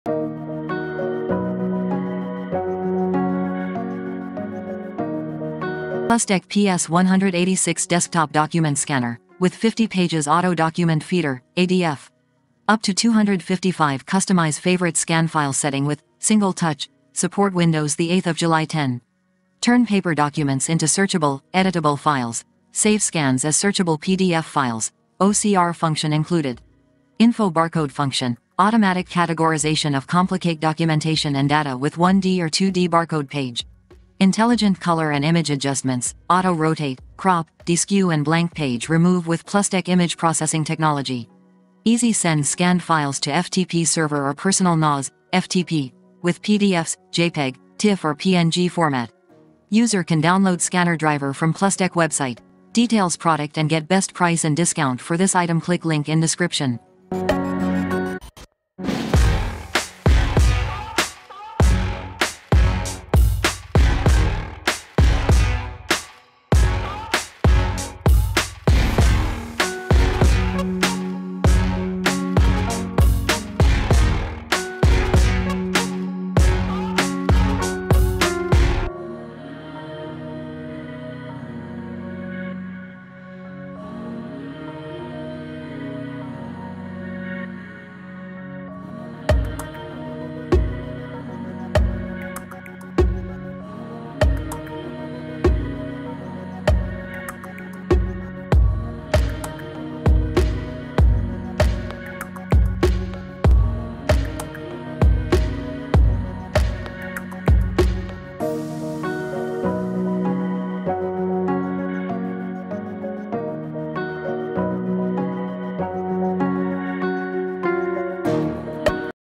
Plustek PS186 Desktop Document Scanner, with 50-pages Auto Document Feeder (ADF). For Windows
Plustek PS186 Desktop Document Scanner, with 50-pages Auto Document Feeder (ADF). For Windows 7 / 8 / 10 / 11 (Intel/AMD only).

Features smart clipboard, you can crop any text or image directly from the scanned image. It can be used when you only need part of the document, and quickly re-use the content/image.